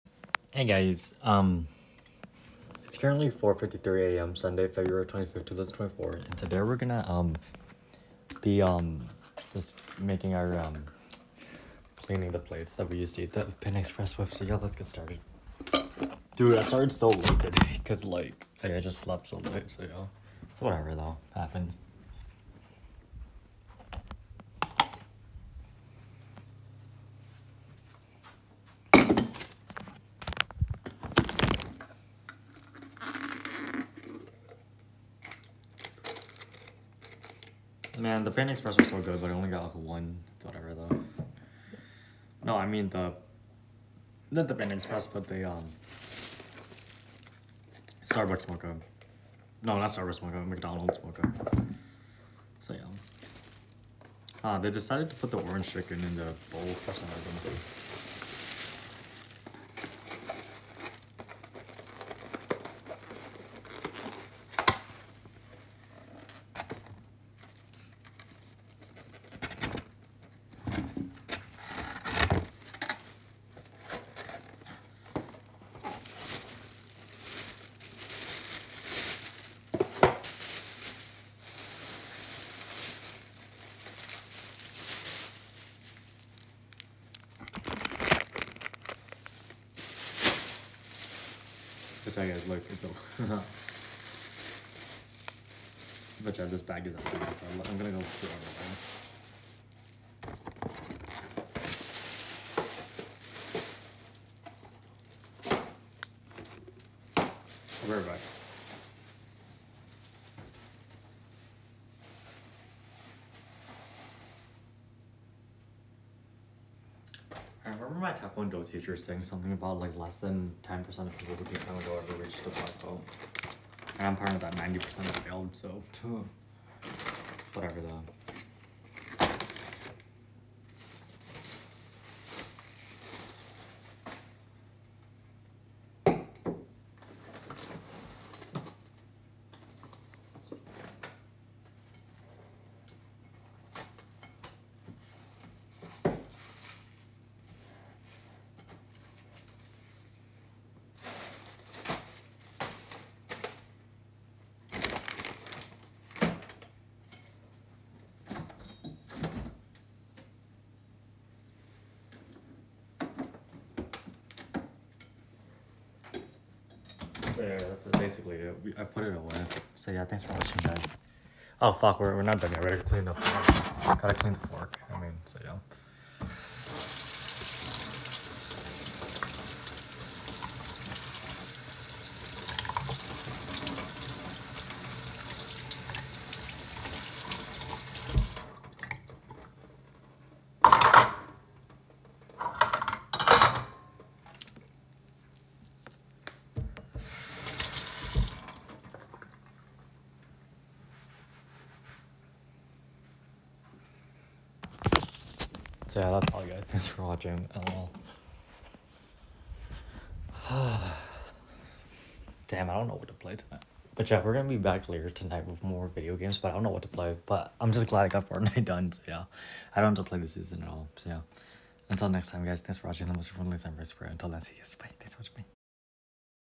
IRL STREAM CLEANING THE PLATES I USED TO EAT MY MUKBANG DINNER OF PANDA EXPRESS WITH
IRL STREAM START TIME AND DATE: 04:53 AM ON 02/25/24
IRL STREAM END TIME AND DATE: 04:56 AM ON 02/25/24
I'm glad I didn't procrastinate this time around -- Watch live